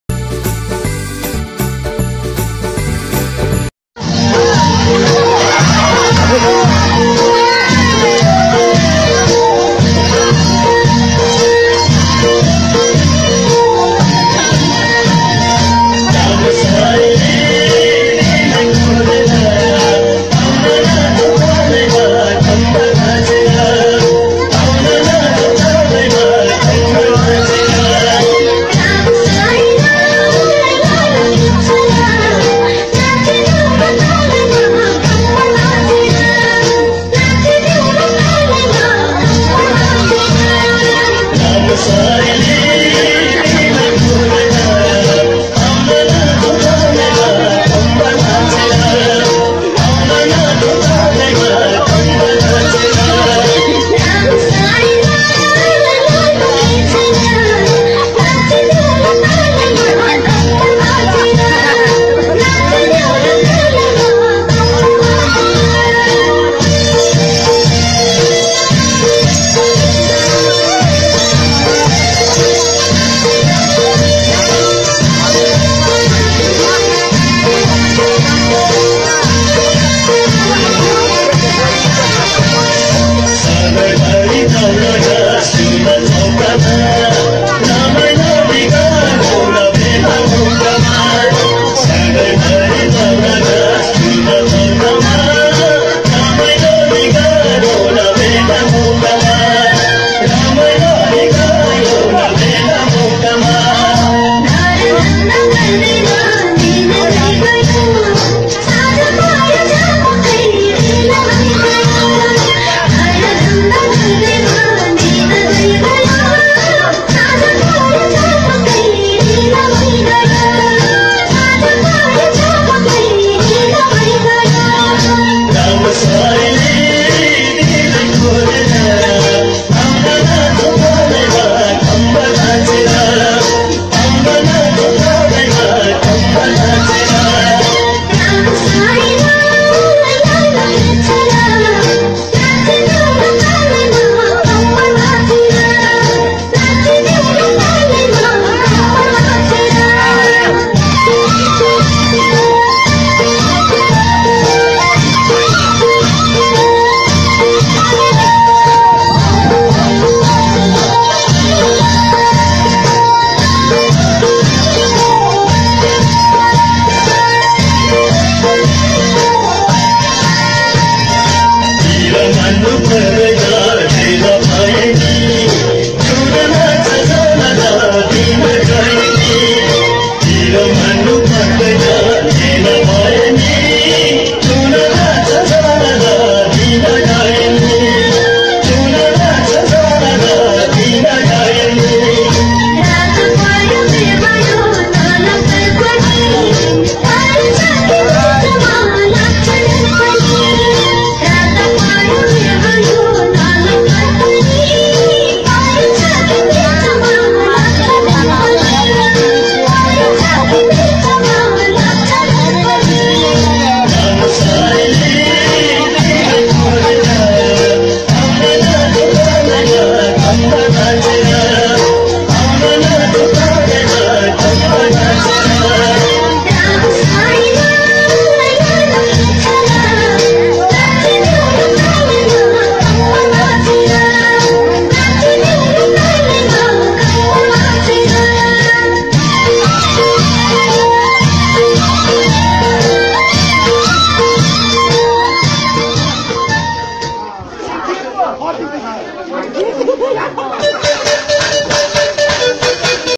दुई दिनको जीवन मरिजाने चोला आज छ भोलि नहोला:रमाइलो गरेर गाउँघरकै शोभा राख्छन बृद्धाबुबाहरुले
नाच्ने गाउने रमाइलो गर्ने मनहरु आखिर बाचुन्जेलत होनि मरेर गए पछि को नै नाच्न गाउन सक्छर?घाम छाया जस्तै मानिसको दुई दिने चोला आज होला भोलि नहोला कसले पो देखेको छर?जस्तै दुख वेदना भए पनि दिलखोलेर रमाइलो गर्नु हुनुहुने बृद्धाबुबाहरु सबैलाई सम्मान छ!🙏🙏

                            धन्यवाद ।।।